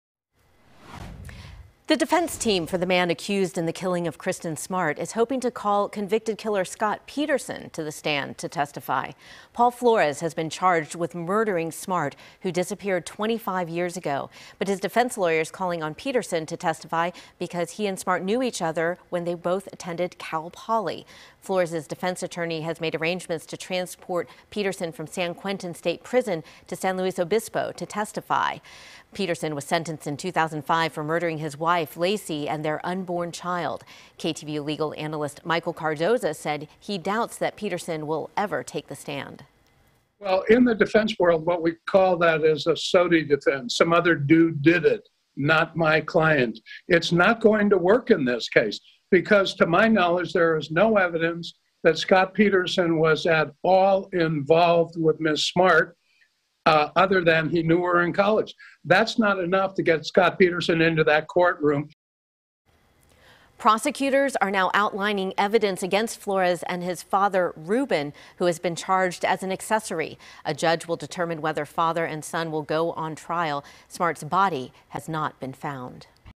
Defense team calling for Scott Peterson to testify in Kristin Smart's murder
The defense team for Paul Flores, who is on trial for the murder of Kristin Smart, is calling for Scott Peterson to testify in Smart's disappearance. Flores argues that Peterson knew Smart, and was even at the same party as Smart before she went missing.